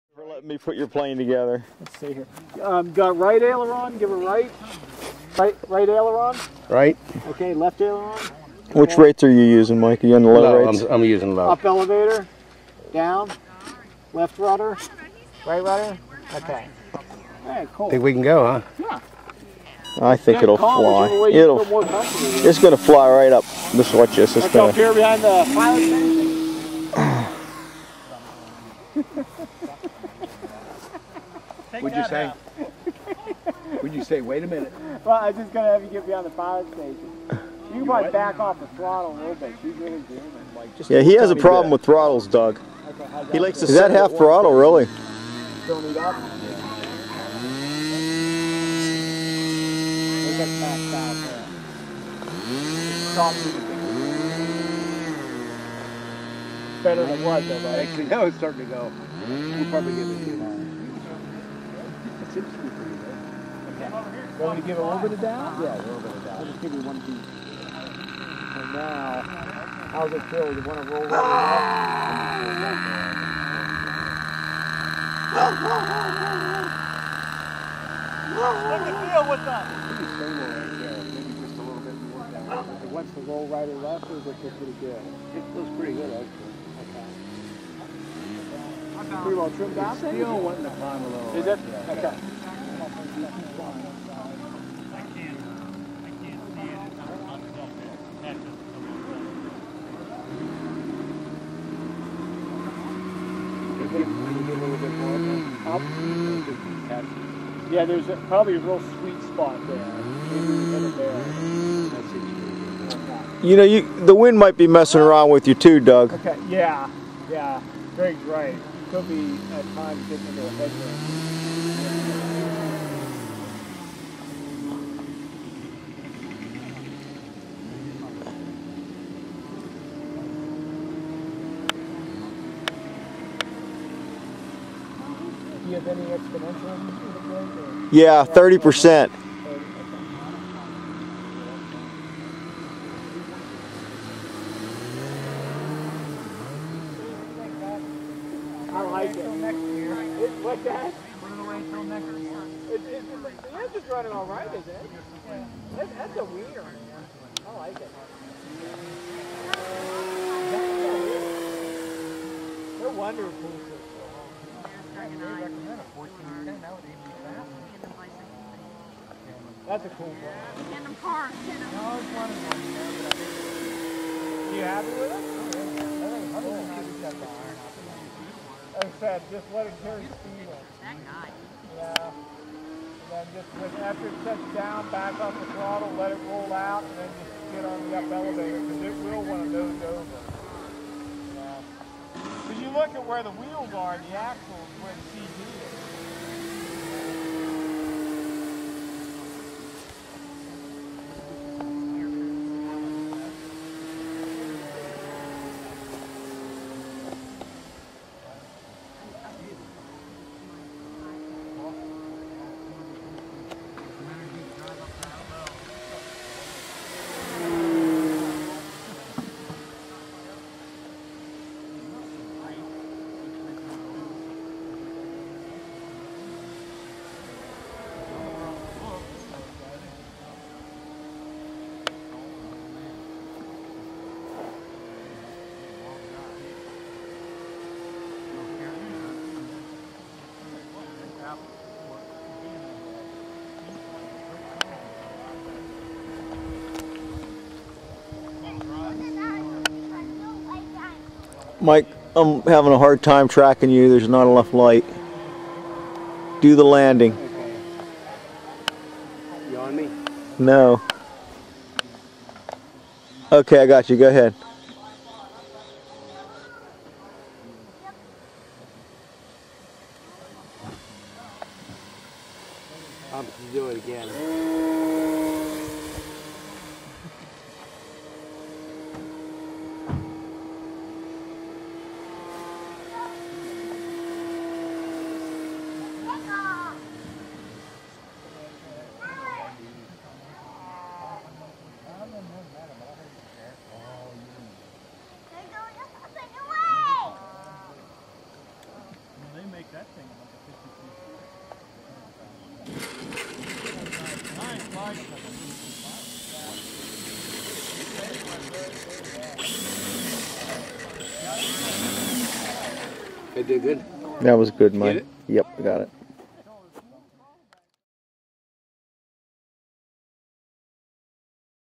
Great Planes YAK 55 Maiden Flight
Mike Krapf flies the maiden flight of his new YAK 55.